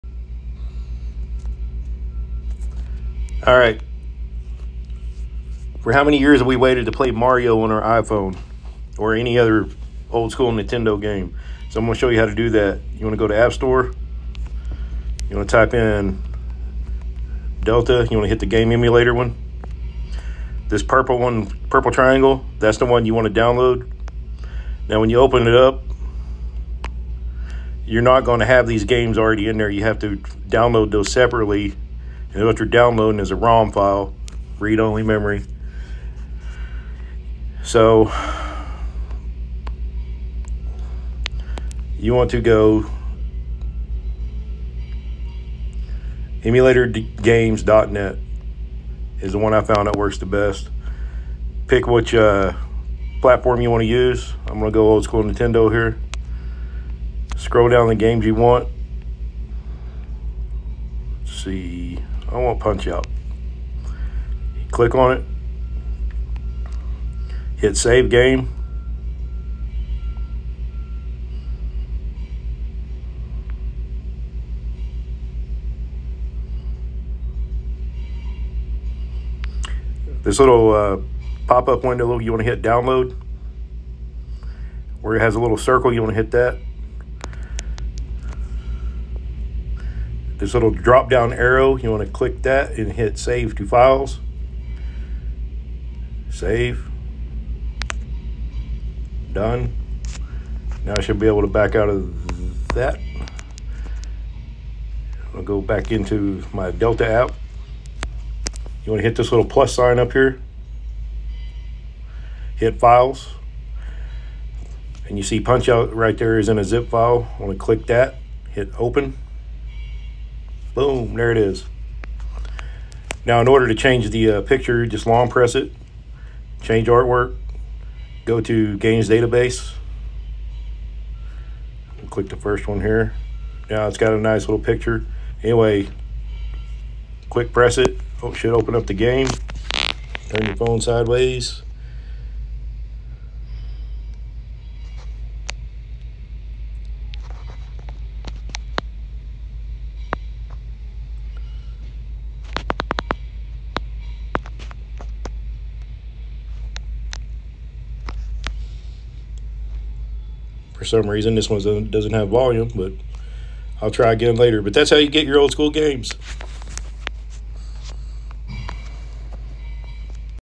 How to play Nintendo games on iPhone.
Nintendo emulator for iOS
Delta app for iOS
Download Nintendo game ROM’s